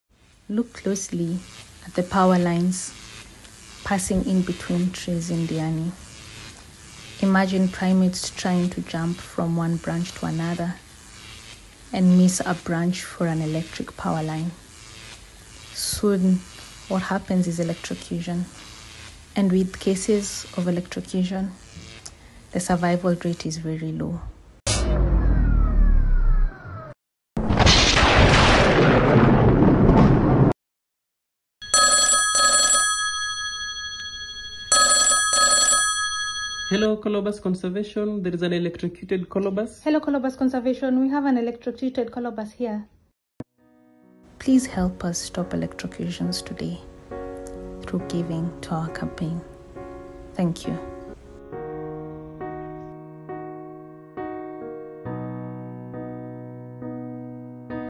Stop the electrocution of colobus monkeys video
Dear Friends.
This week alone, we have had two electrocution incidents. The problem we have at hand is not only a Diani problem but a worldwide problem. The solution to this electrocution problem lays in your hands. You, our supporters, have the ability to cause change at a local and global level. By aiding us in the insulation of power lines in Diani, you directly contribute to the prevention of further tragic deaths due to electrocution. Additionally, your support enables us to share our hard-earned insights with conservation organizations worldwide, amplifying the impact of our collective efforts.
We are grateful to you because, through your online and offline donations, we have $1,858 remaining to reach our goal. Consider donating to us today. While your donation may seem modest to you, the impact it can have transcends its monetary value. Together, let us create safer pathways for wildlife.